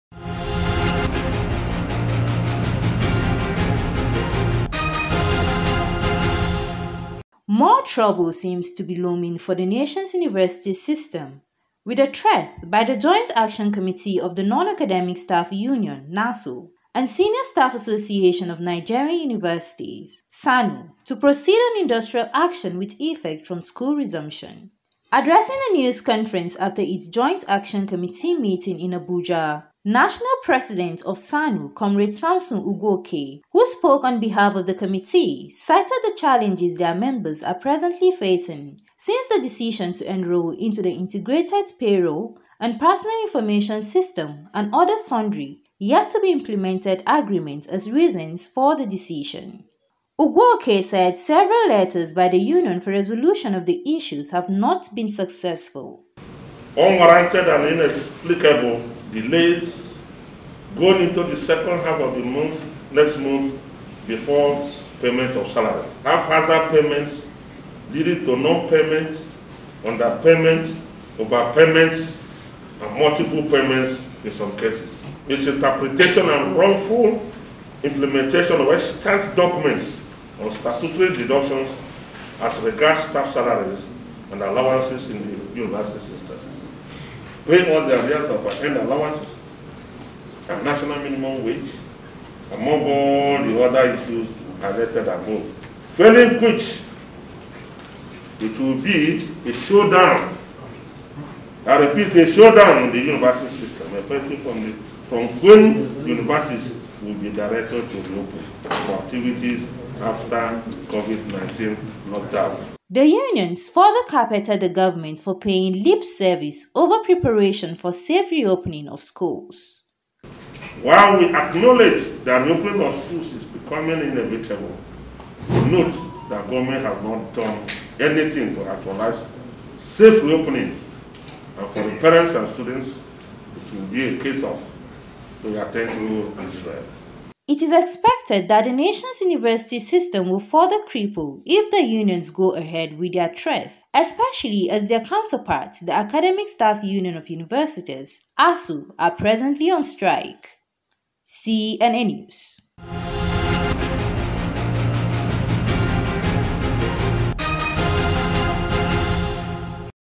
SSANU,NASU Threaten Strike Over IPPIS,Others.
More trouble seems to be looming for the nation's university system with the threat by the joint action committee of the non academic staff Union (NASU) and Senior Staff Association of Nigerian Universities to proceed on industrial action with effect from school resumption.

Addressing a news conference after its joint action committee meeting in Abuja, National President of SSANU Comrade Samson Ugwoke who spoke on behalf of the committee, cited the challenges their members are presently facing since the decision to enroll into the Integrated Payroll and Personnel Information System (IPPIS) and other sundry yet to be implemented agreement as reasons for the decision.

Ugwoke said several letters by the union for resolution of the issues have not been successful.

The unions further carpeted the government for paying lip service over preparation for safe reopening of schools.

It is expected that the nation's university system would be further crippled if the unions go ahead with their threat especially as their counterparts, the academic staff Union of universities ASUU are presently on strike.